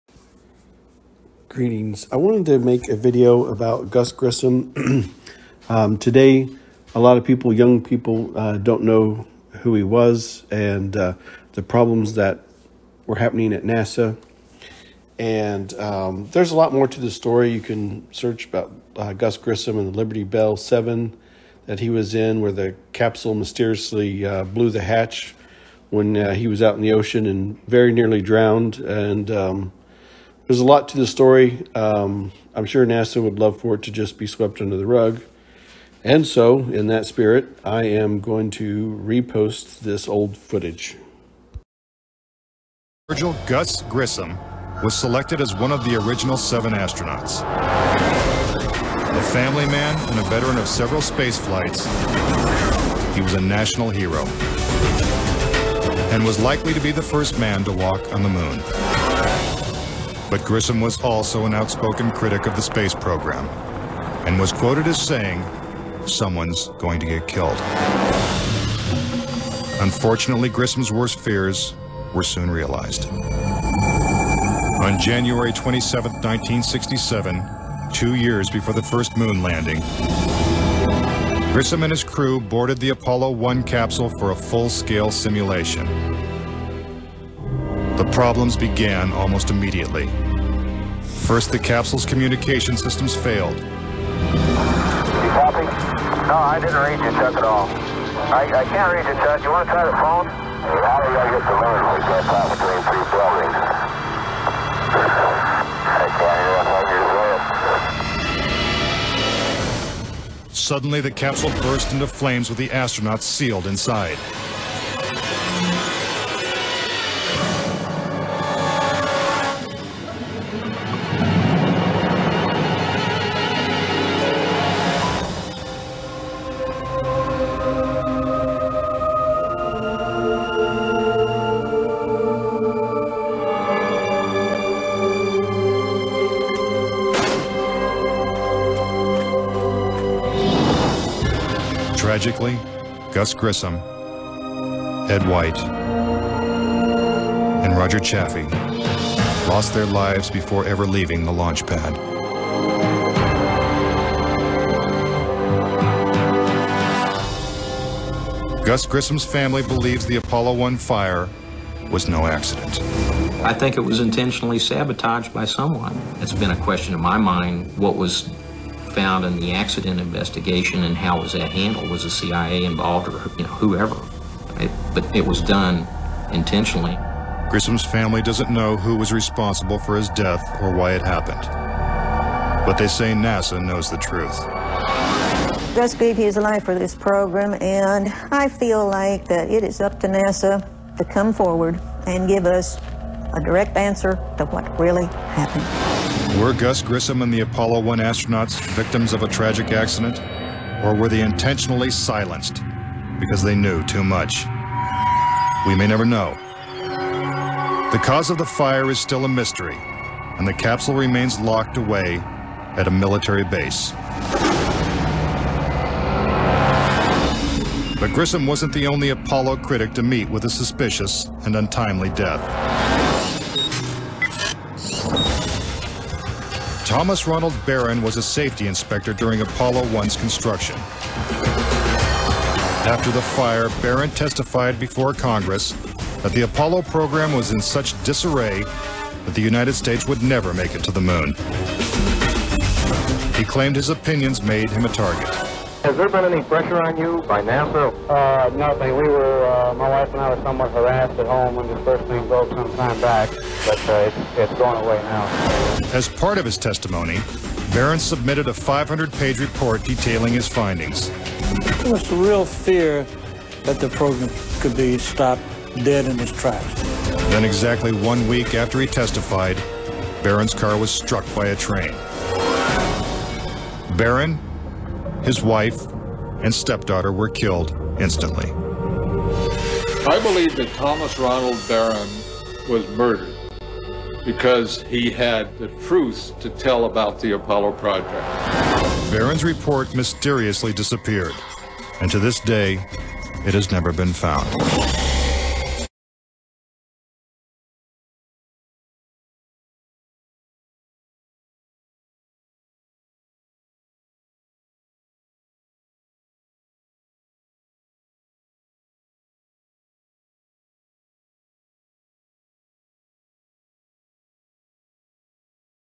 Gus Grissom and the Problems at NASA
A look at an old report and problems at NASA